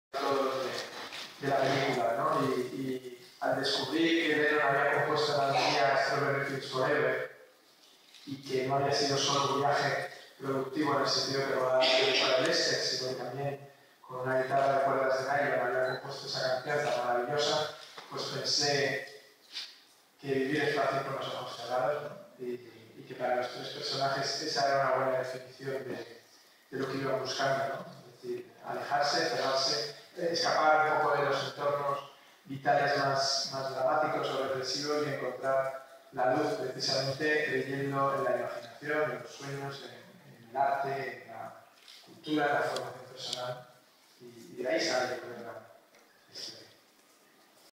Vivir es fácil con los ojos cerrados, de David Trueba
Título: Vivir es fácil con los ojos cerrados, de David Trueba

Descripción: Pase de prensa de `Vivir es fácil con los ojos cerrados` de David Trueba, en Museo Tyssen Bornemisza, día 29 octubre 2013, a las 12:30 horas. Con la presencia de David Trueba, Javier Cámara y Natalia de Molina.

Fecha: 03/11/2013

Materia: Ciencias de la Información

Submateria: Cine

Formato: MPEG-2

Fuente: Complumedia-RTVDoc

Idioma: Español